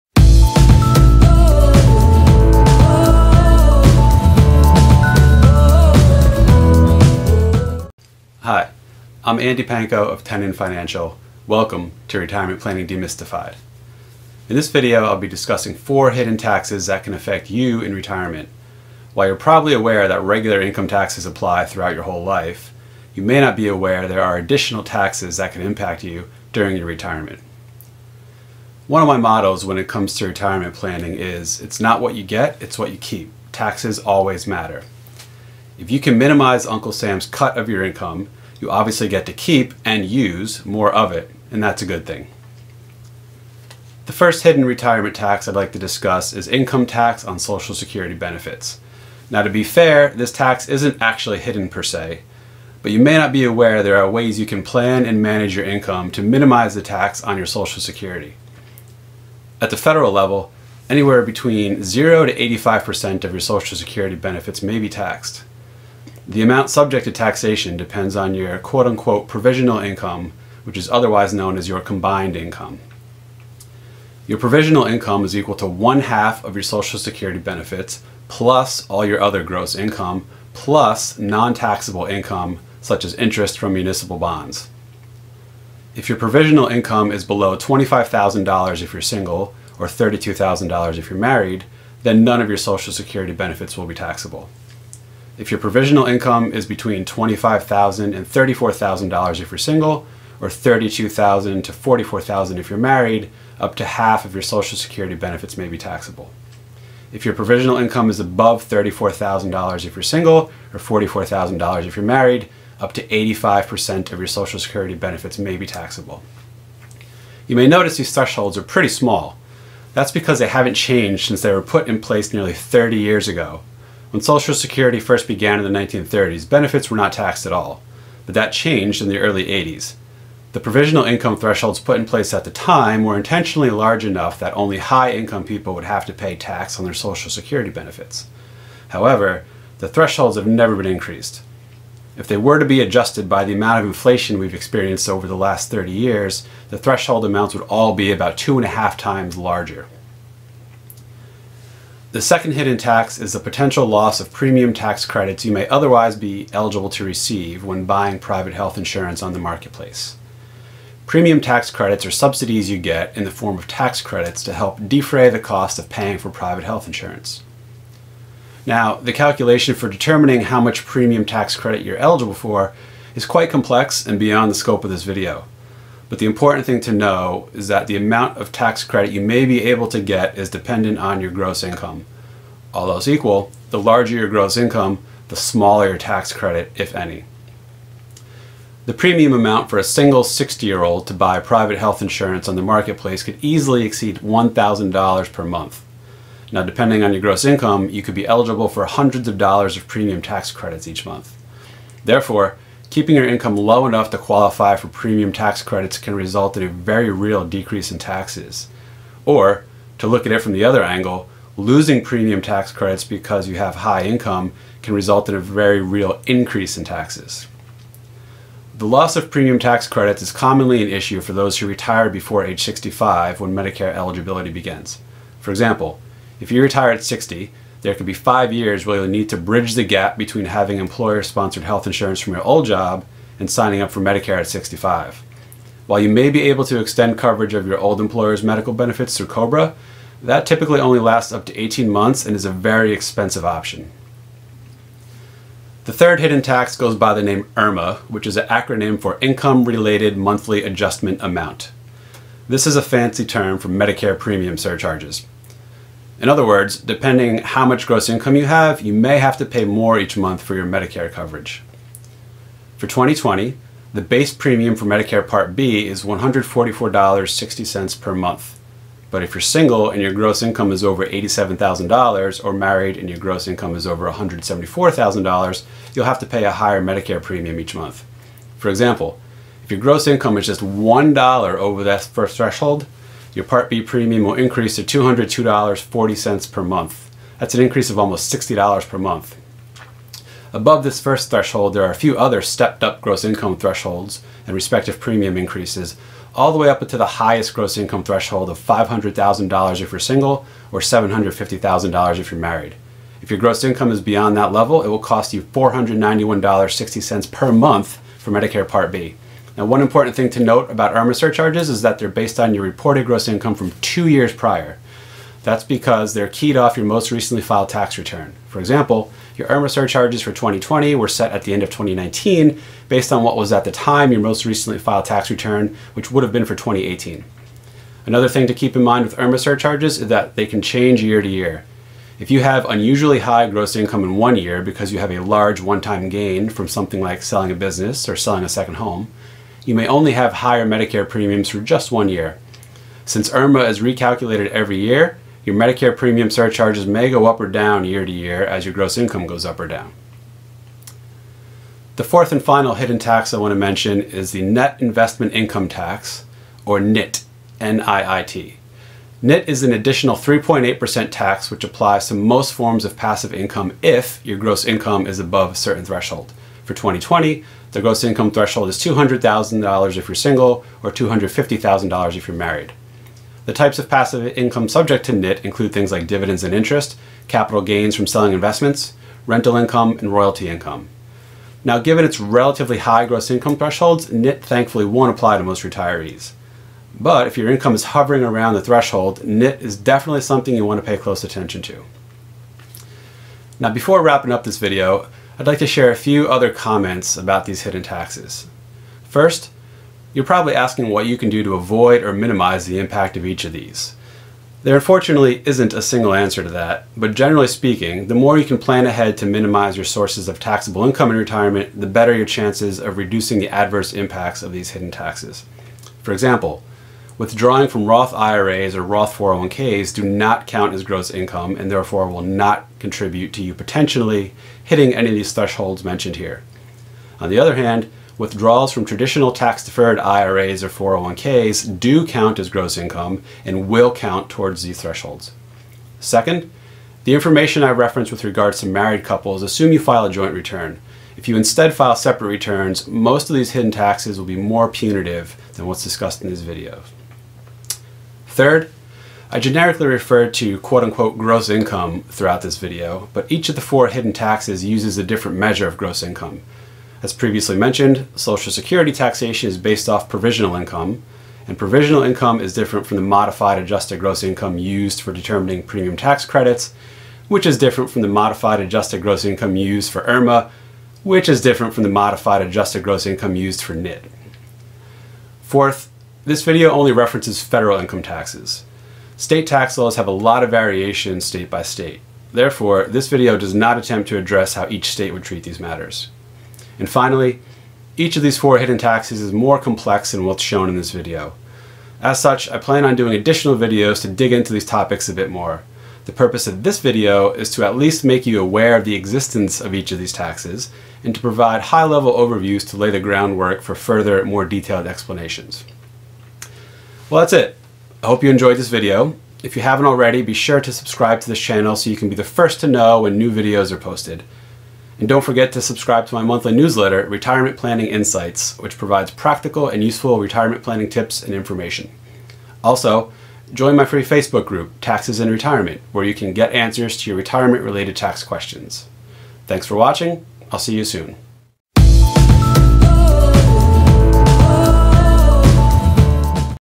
Hidden Taxes in Retirement
Overview of four hidden taxes you may face in retirement: 1) taxation on Social Security benefits, 2) health insurance premium tax credits, 3) Medicare premium surcharges (aka IRMAA or Income Related Monthly Adjustment Amount) and 4) NIIT or Net Investment Income Tax.